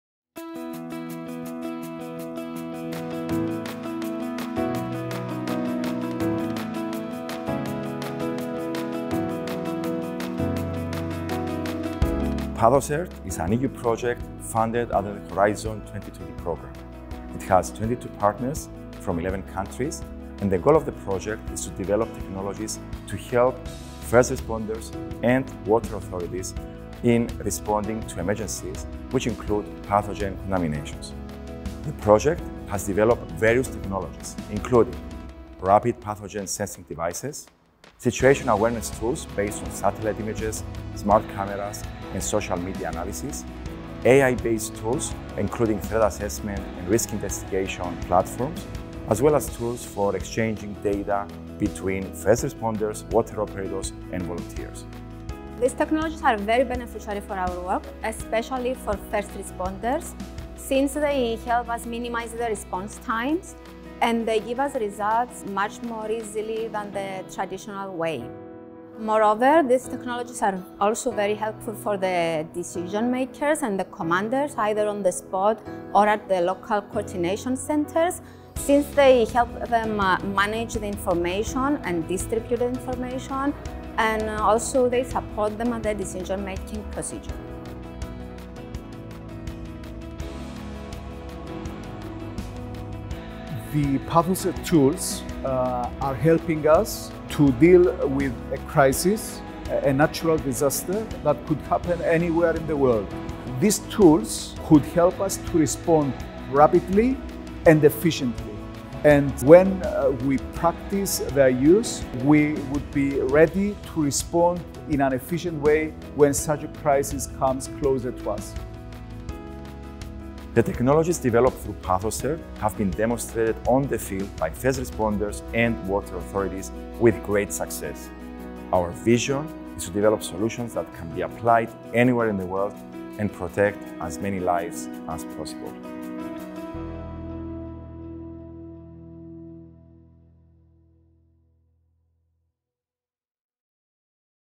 H2020 PathoCERT Limassol Pilot
Video of the H2020 PathoCERT Limassol Pilot, coordinated by the Cyprus Civil Defense and the University of Cyprus, together with the Water Development Department, the Water Board of Limassol and the Sewarage Board of Limassol-Amathus.